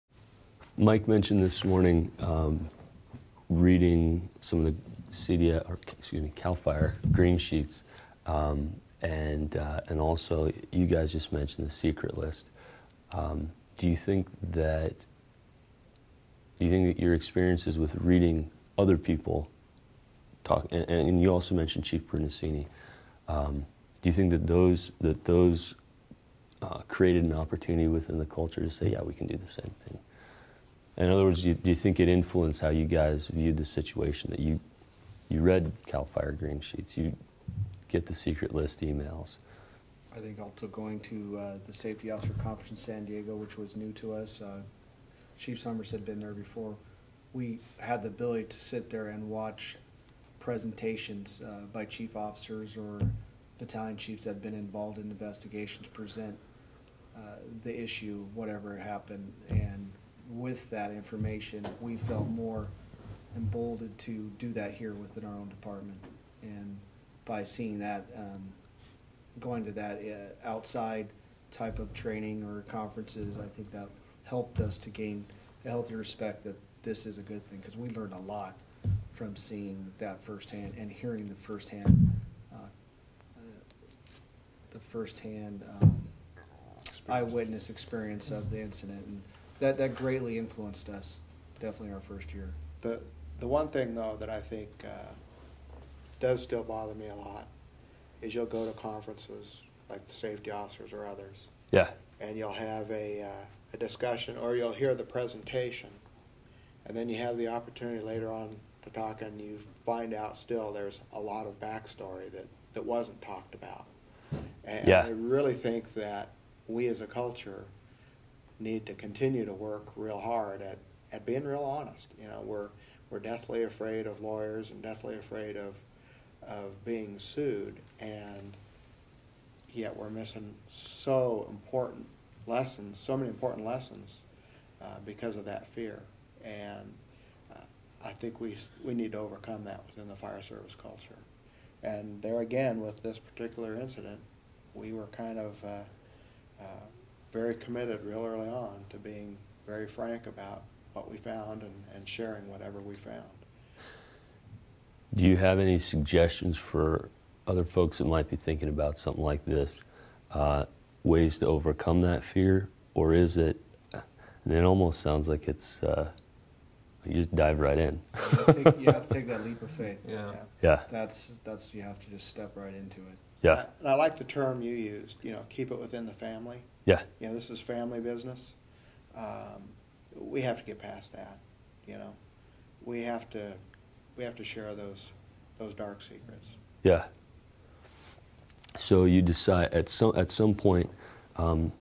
Chino Valley Clip 7
A discussion about how outside stories affected this investigation